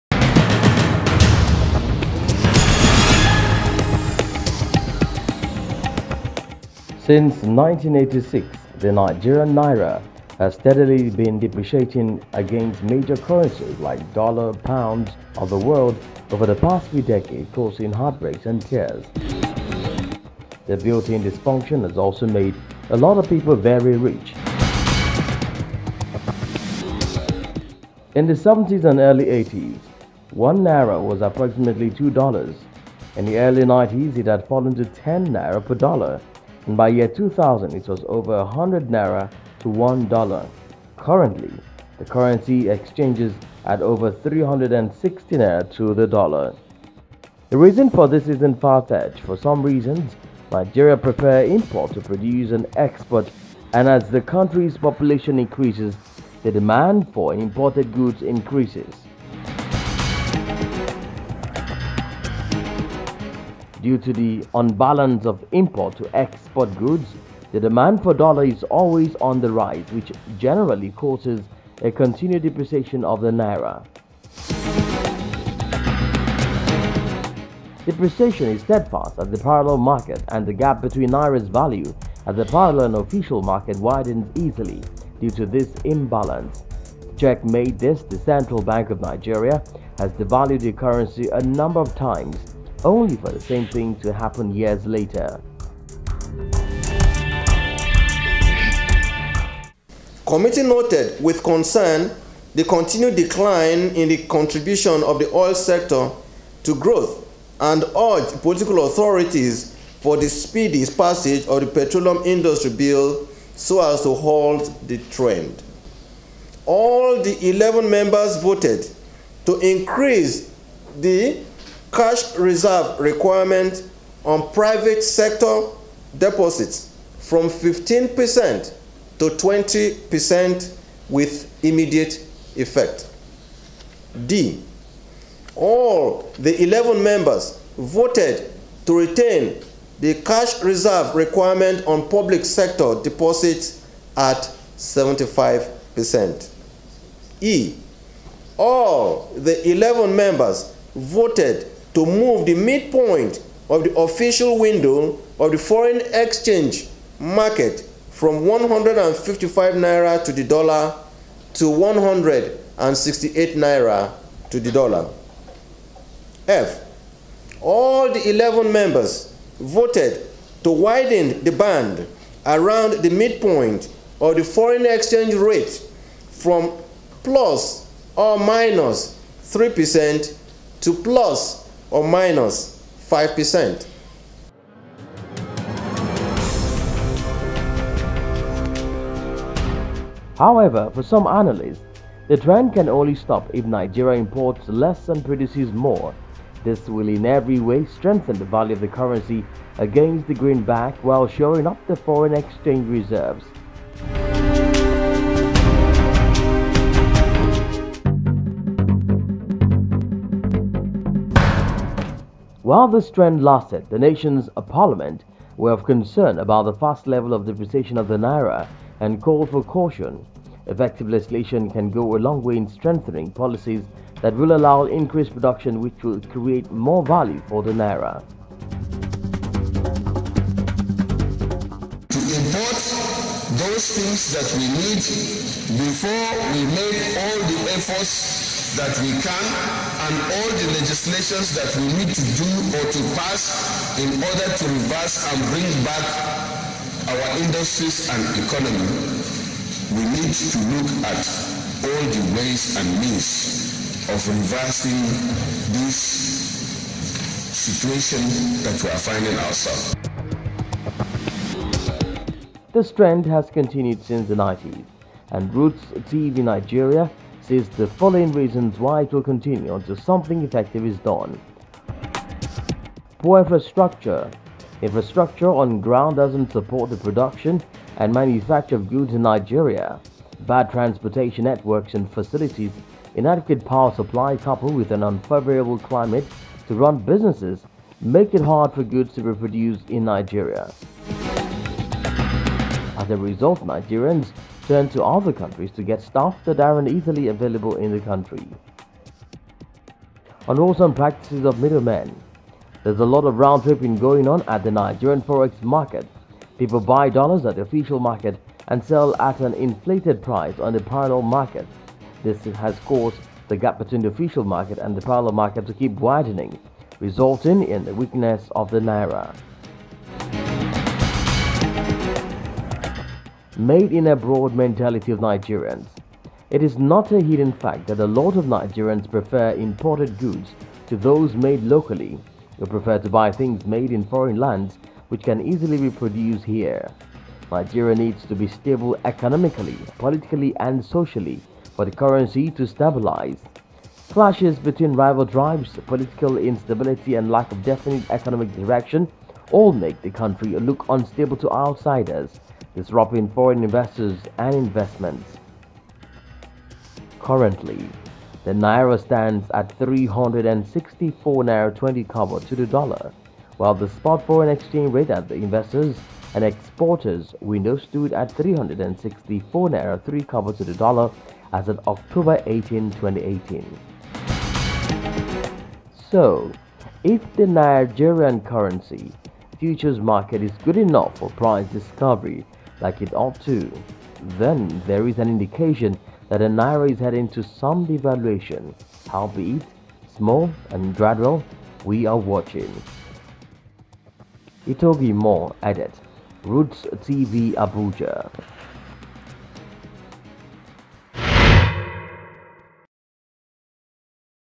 The Naira : Story Of Rise And Fall
Since 1986 the Nigerian Naira, has steadily been depreciating steadily against major currencies (dollar, pounds, etc) of the world over the past few decade, causing heartbreak and tears. The built-in dysfunction has also made a lot of people very rich.
In the 70’s and early 80’s one Naira was approximately two dollars . In the early 90’s it had fallen to N10 per dollar and by year 2000 it was over a hundred Naira to one dollar. Currently, the currency exchanges at over 360 to the dollar.